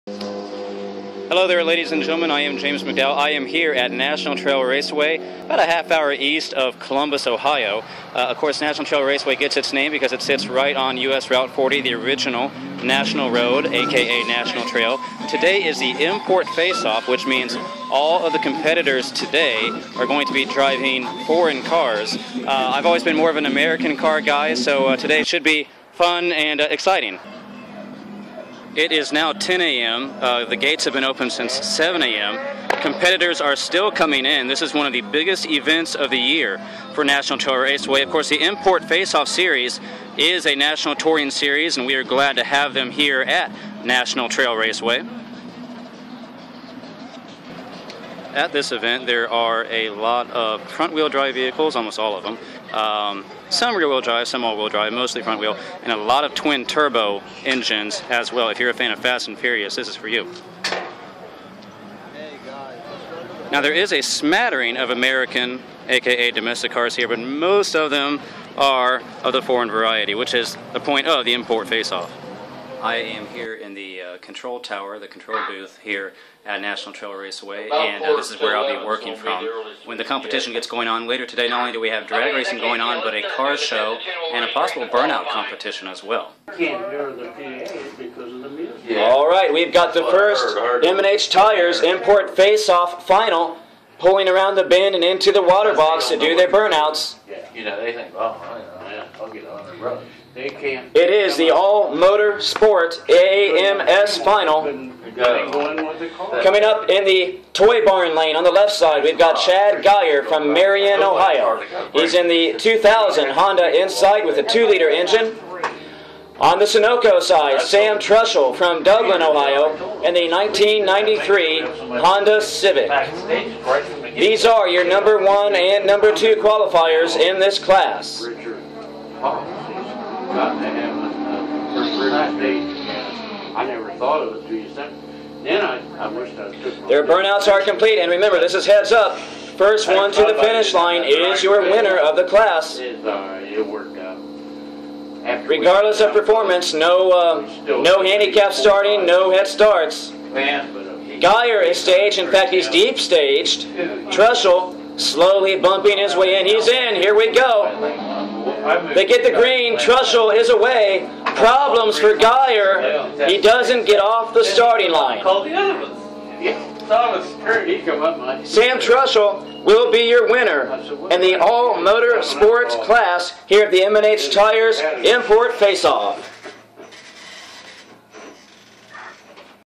A Day at NTR
Last Sunday, the Import Face Off series came to National Trail Raceway in Hebron, Ohio. I had the privilege to work as the announcer for the event. Here's a behind the scenes look!